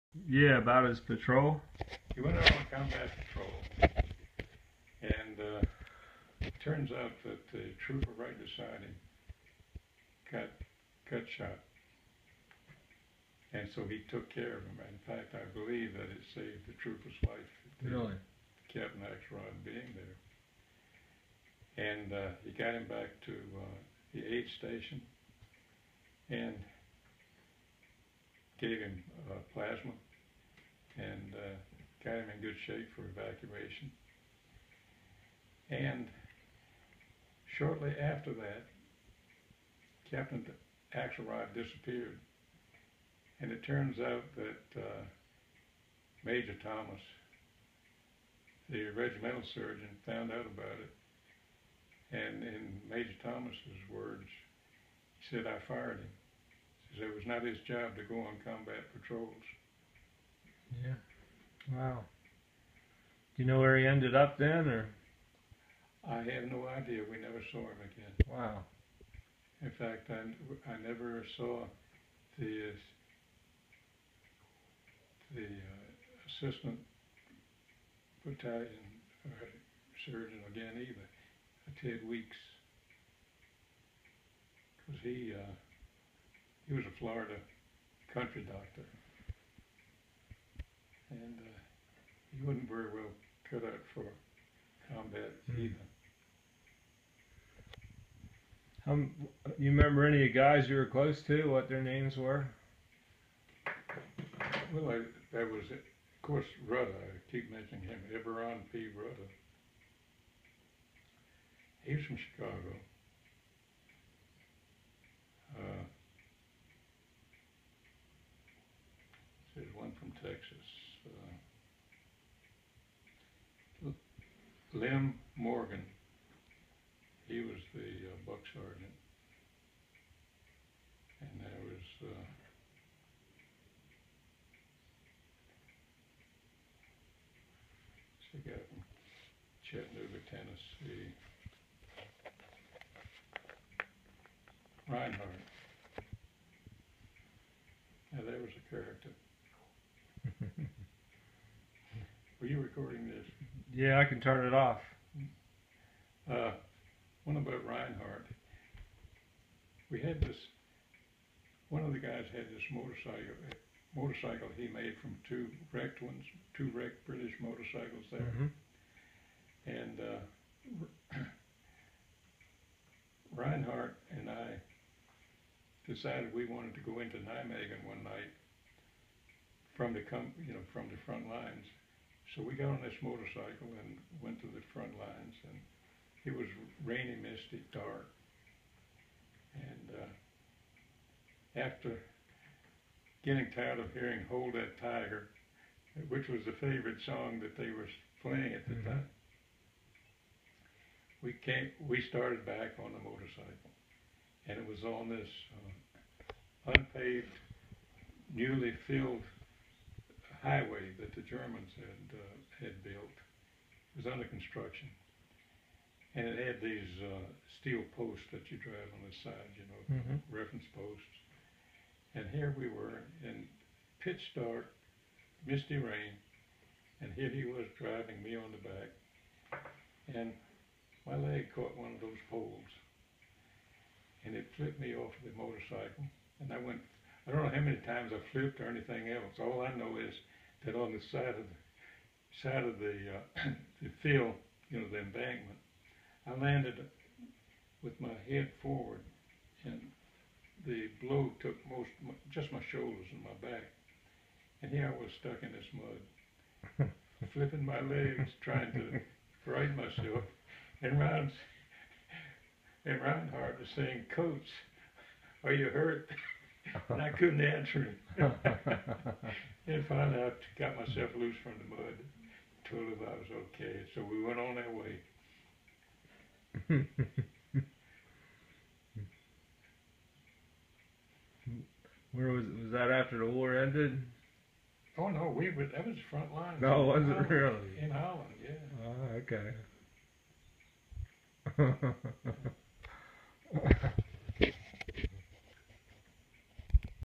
John Coates 508 PIR 82nd Airborne medic WW2
Talking about his time in 1944 Holland.